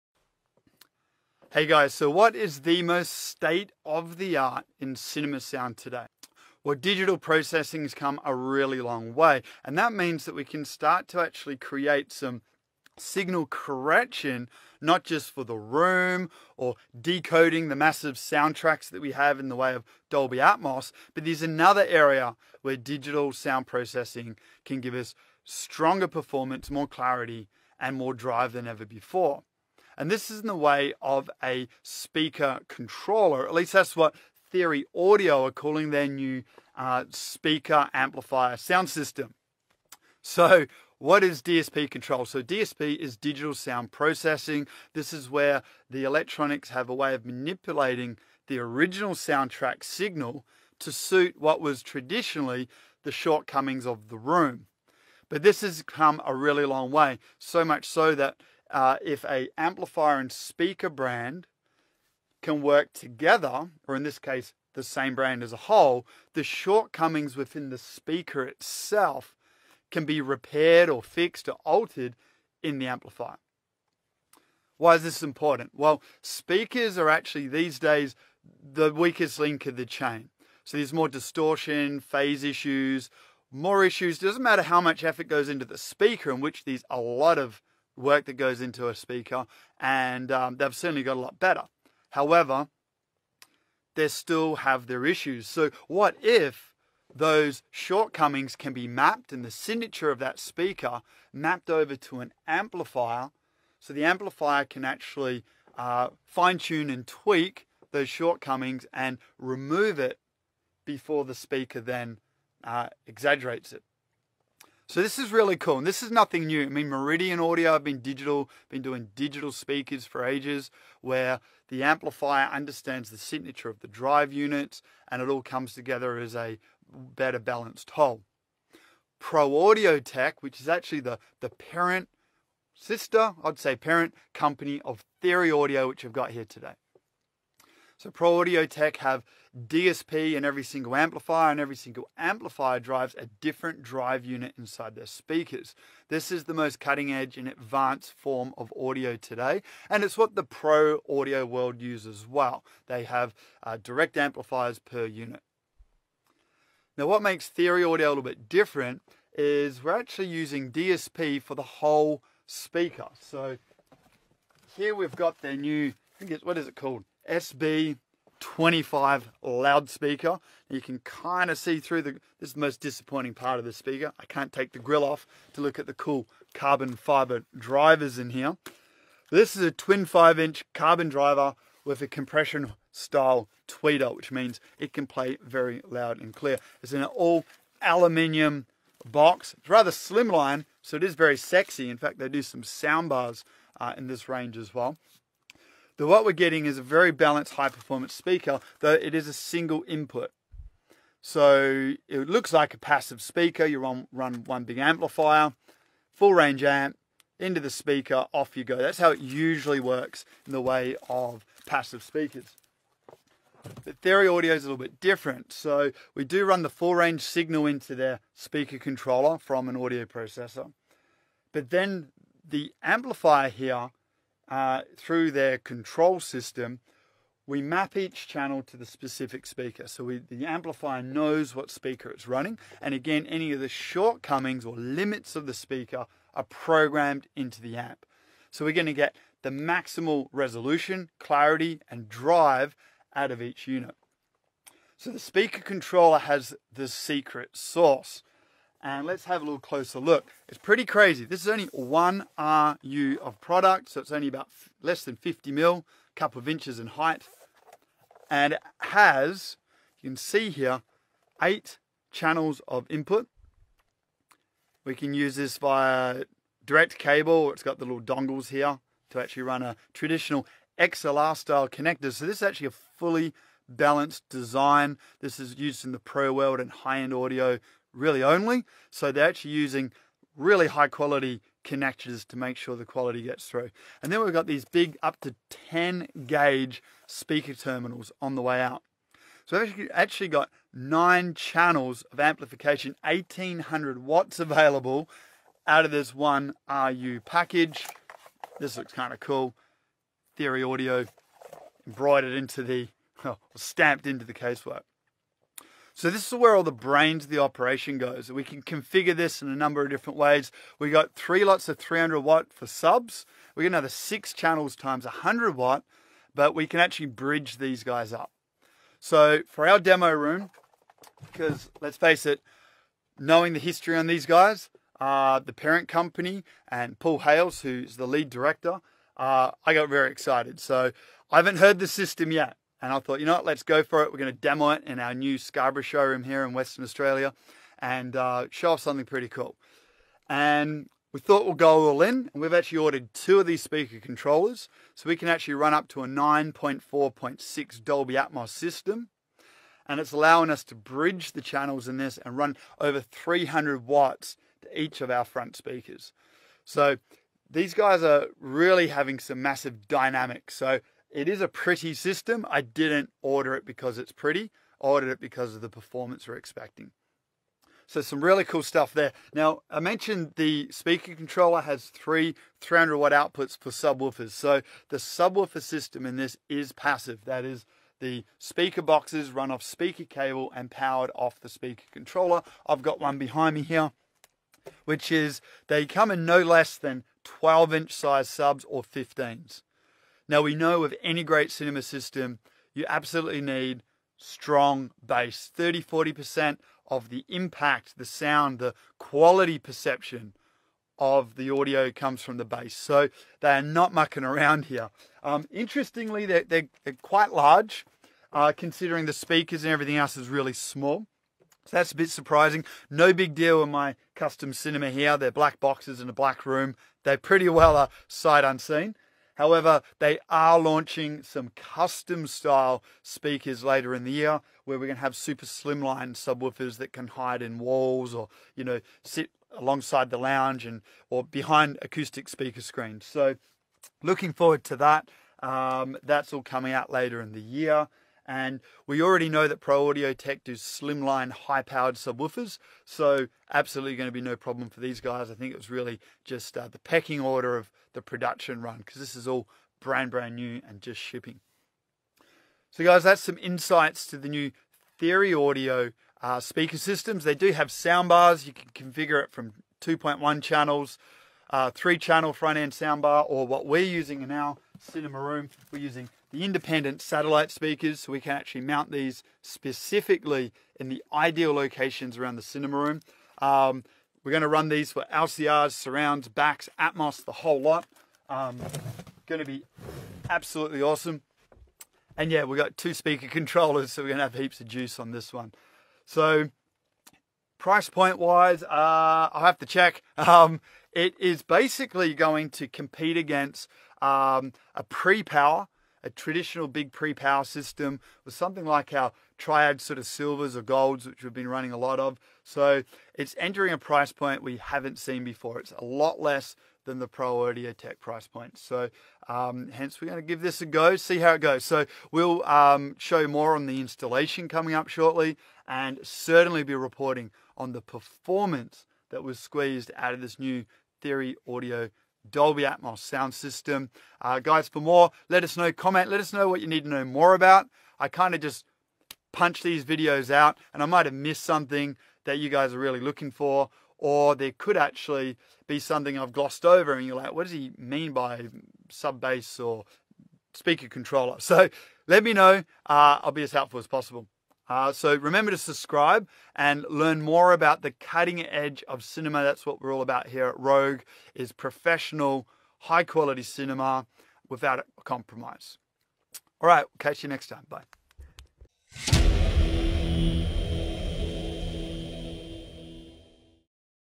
Theory Audio 1800 watts Cinema Sound System
A look at Theory Audios 1800 Watt Cinema Sound System